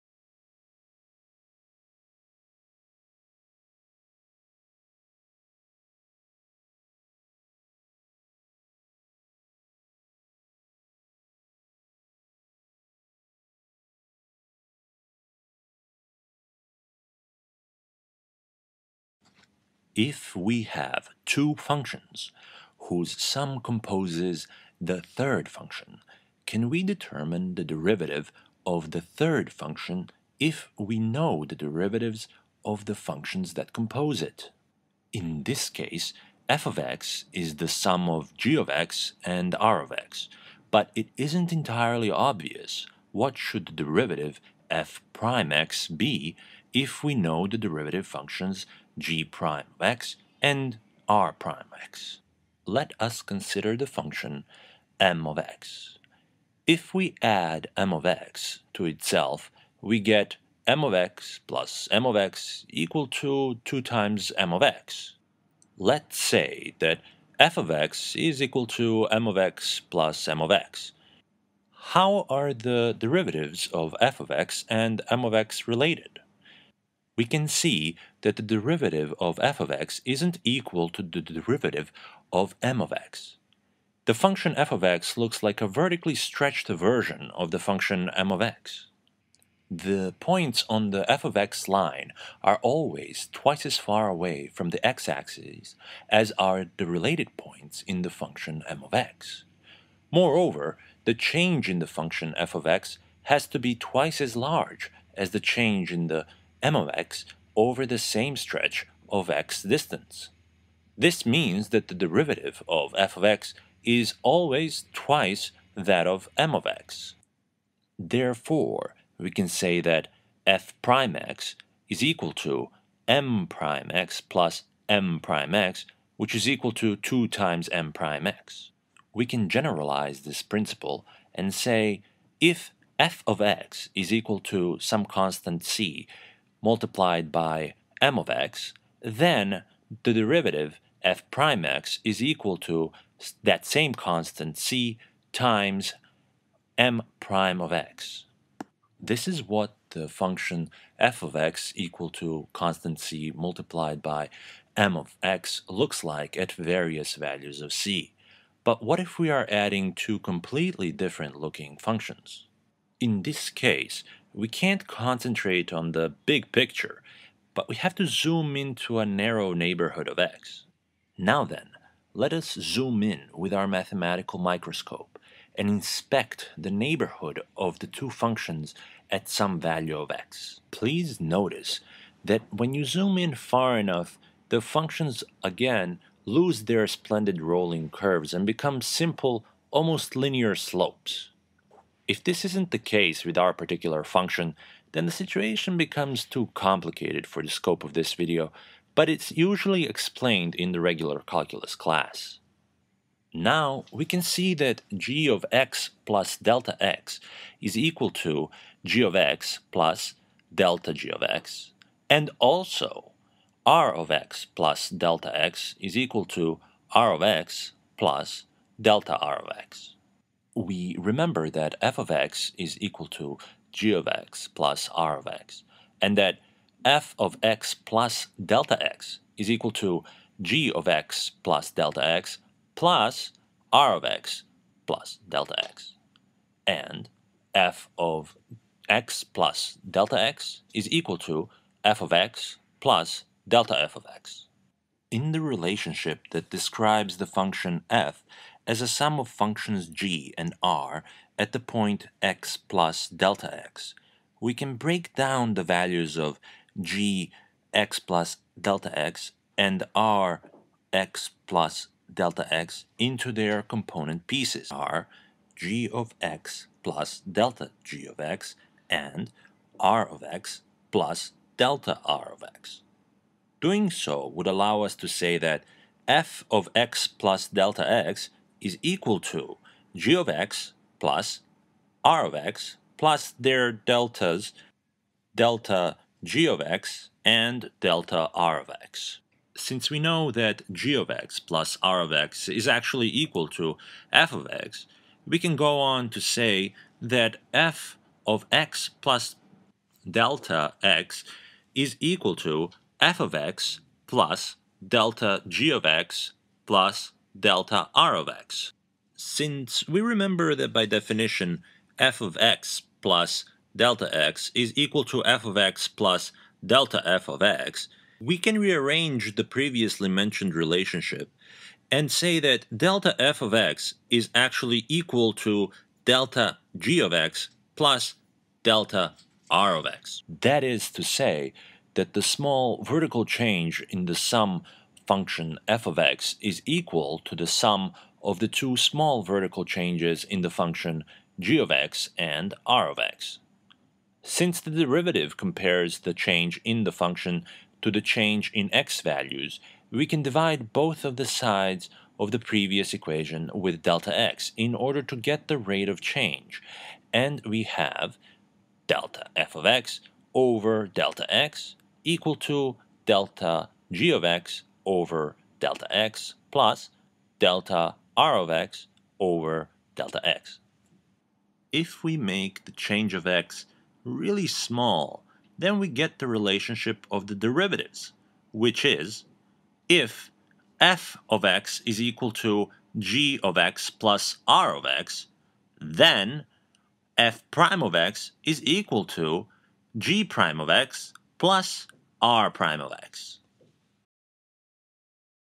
Derivatives of Addition - A Calculus Math Tutorial
we show a derivative of a function that is composed of two added functions is explained through geometry. This short but very informative tutorial presents a relatively simple calculus concept in an intuitive visual fashion.